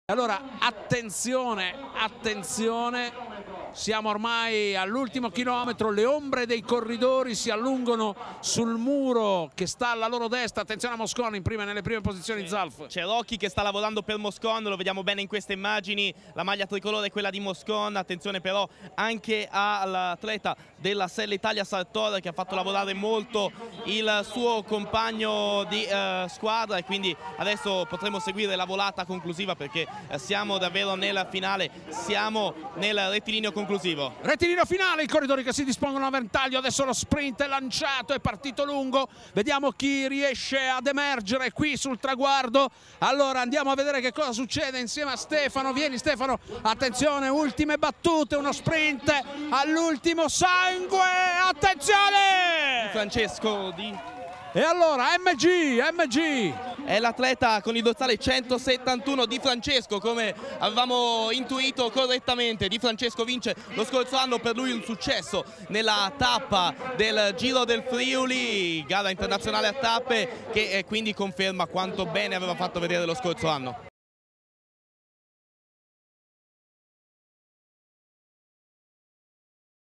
Trofeo Città di Brescia 2015 | vittoria di Gian Marco Di Francesco
ORDINE DI ARRIVO:
1° DI FRANCESCO GIAN MARCO (MG.KVIS VEGA NORDA)
2° MAESTRI MIRCO (GENERAL STORE BOTTOLI ZARDINI LIOTTO)
3° TIZZA MARCO (TEAM IDEA 2010 ASD)
4° BARBERO PIANTINO MASSIMILIANO (TEAM CERONE-RODMAN-RAFI)
5° MARTINELLI DAVIDE (TEAM COLPACK)
6° SARTOR FEDERICO (SELLE ITALIA-CIEFFE-URSUS)
7° PACCHIARDO DAVIDE (TEAM PALA FENICE ASD)
8° MOSCON GIANNI (ZALF EUROMOBIL DESIREE FIOR)
9° MOSCA JACOPO (L.MASERATI-VIRIS-SISAL MATCHPOINT)
10° BONUSI RAFFAELLO (GENERAL STORE BOTTOLI ZARDINI LIOTTO)